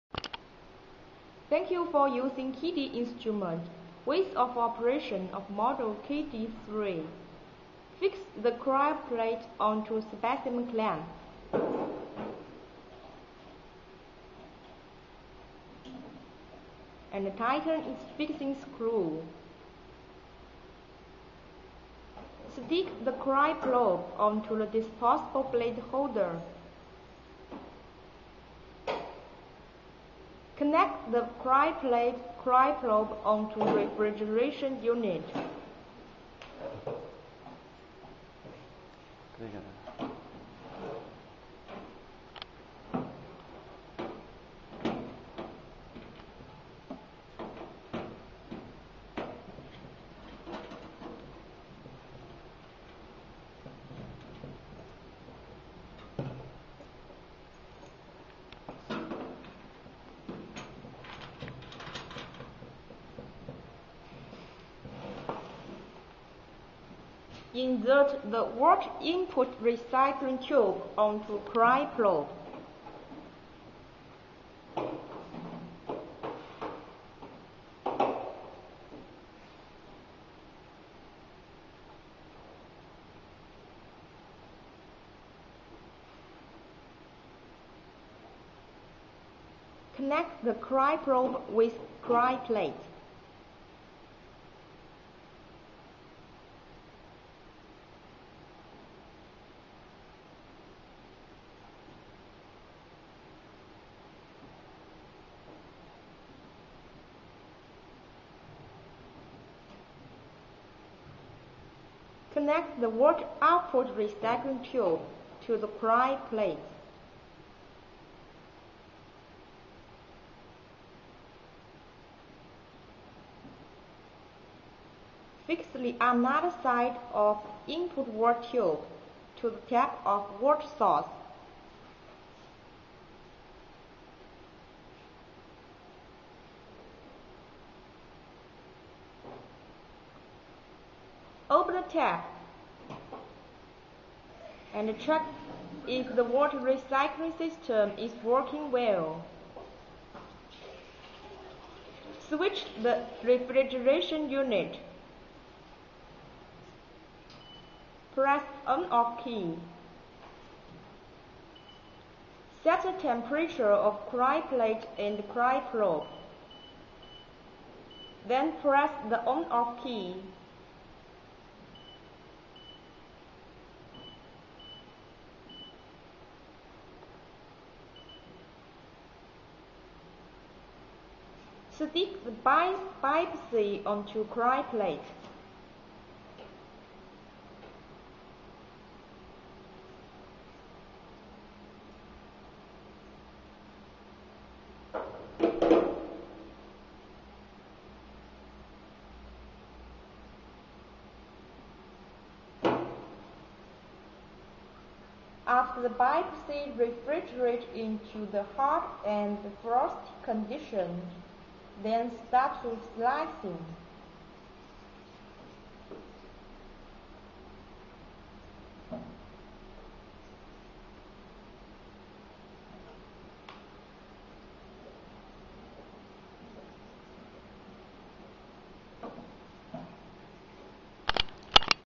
KD-2258 III Manual Microtome -- Dual Purpose for Frozen and Paraffin Sections
KD-2258 III Manual Microtome comes with KD-III Rapid Freezing Mainframe. It is used as dual purpose microtome for both frozen and paraffin sectioning.